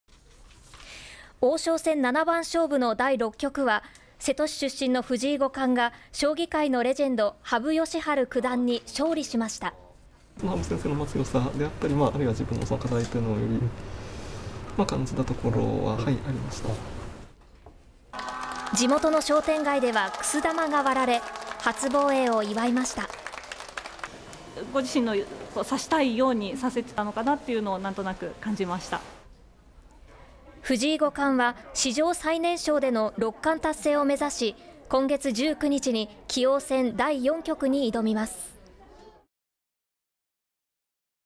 【初防衛】「王将戦」 藤井聡太五冠の地元・瀬戸市で喜び
藤井聡太五冠の地元愛知県瀬戸市では、王将戦の初防衛を受け、喜びに沸きました。

王将戦七番勝負の第6局は、瀬戸市出身の藤井五冠が、将棋界のレジェンド・羽生善治九段に勝利しました。

「羽生先生の強さだったり、自分の課題をより感じたところはありました」（藤井聡太五冠）

地元の商店街では、くす玉が割られ、初防衛を祝いました。

「（藤井聡太さんは）ご自身の指したいように指せたのかなと感じました」（地元の人）

藤井五冠は、史上最年少での六冠達成を目指し、今月19日に棋王戦第4局に挑みます。